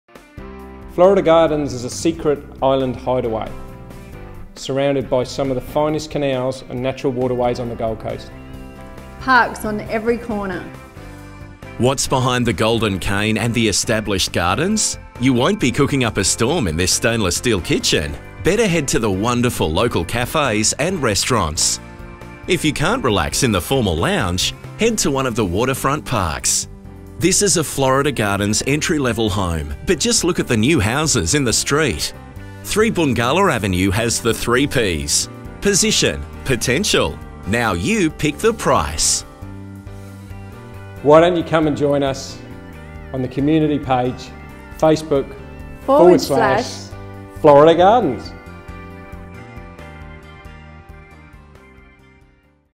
3 Boongala Road, Broadbeach Waters QLD By John Mayer
Property Video shoot of 3 Boongala Road, Broadbeach Waters QLD by PlatinumHD for Ray White Mermaid Beach